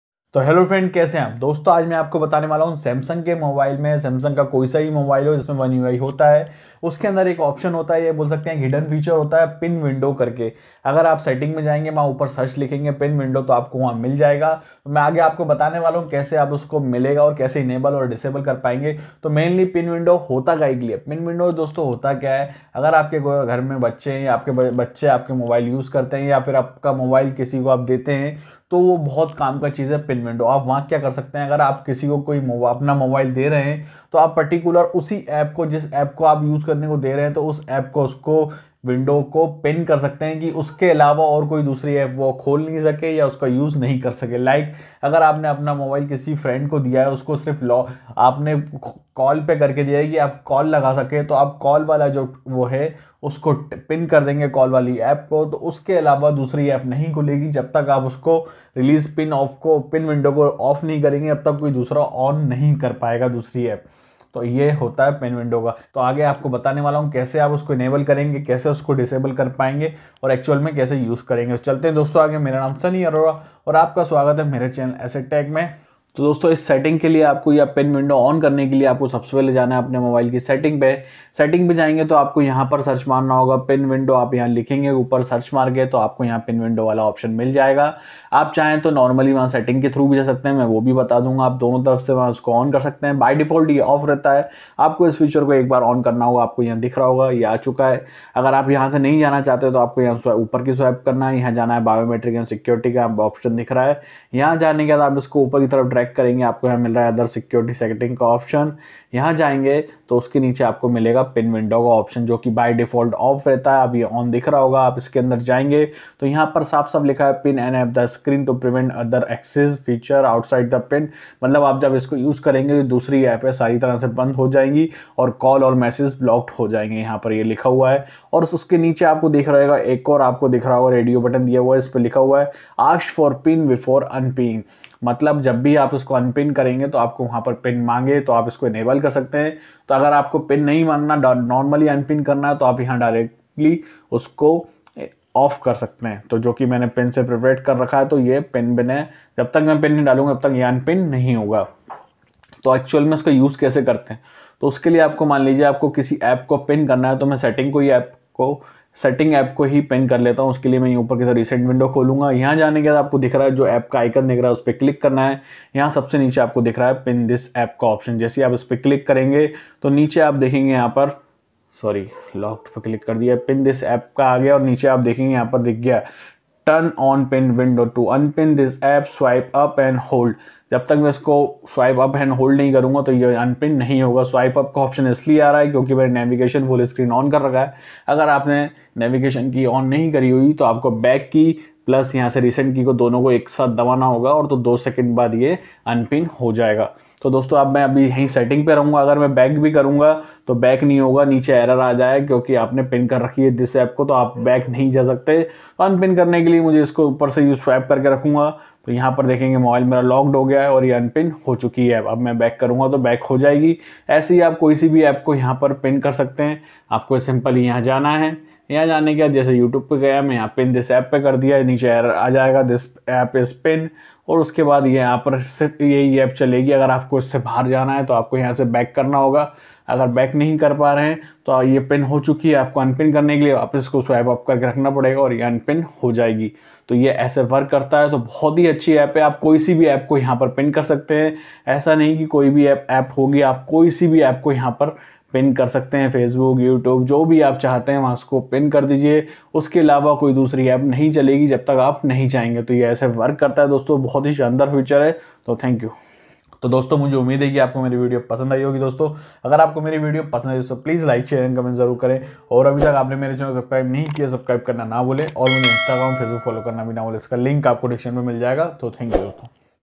How To Enable/Disable Pin Windows Hidden Feature On Samsung A30s,Samsung Pin Windows
In this Video I describe about How to Use Pin Windows Feature on Samsung a30s